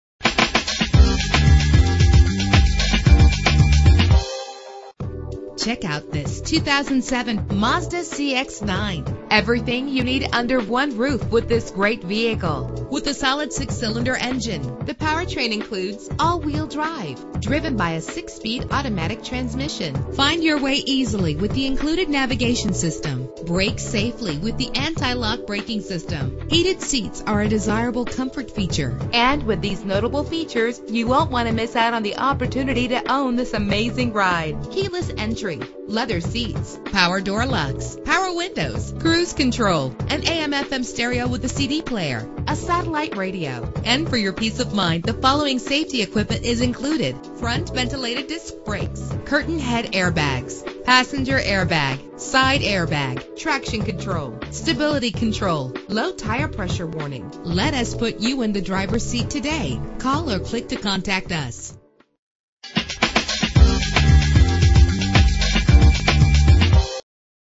2007 Mazda CX-9 - Roseville MN
Year: 2007
Make: Mazda
Model: CX-9
Trim: Grand Touring
Engine: 3.5 liter 6 cylinder 24 valve
Transmission: 6-Speed Automatic
Color: Blue
Mileage: 94352
Address:
2805 Long Lake Rd